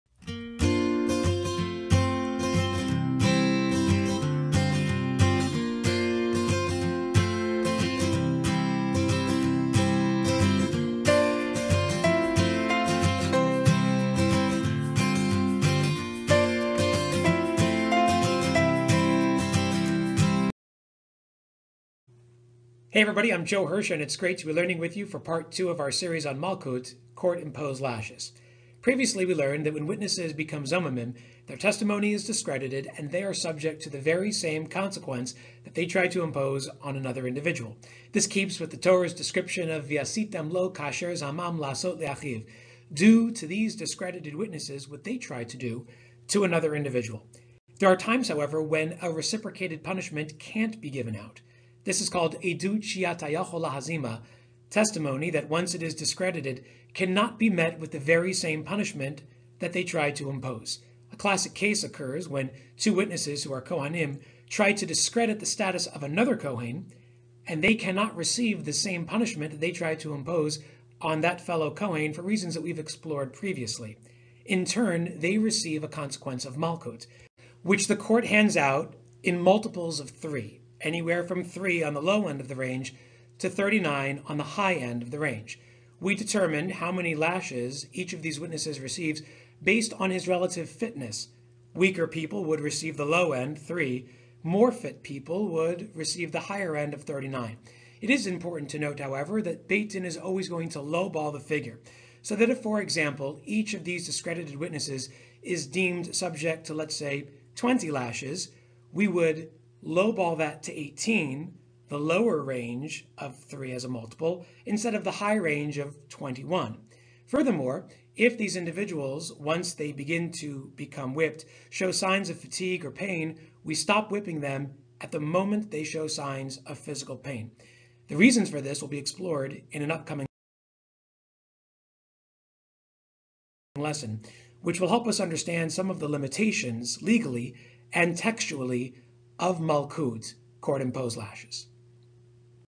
Makkot - Introduction to Malkut, part 2
The second of a four-part series on malkut, court-imposed lashes.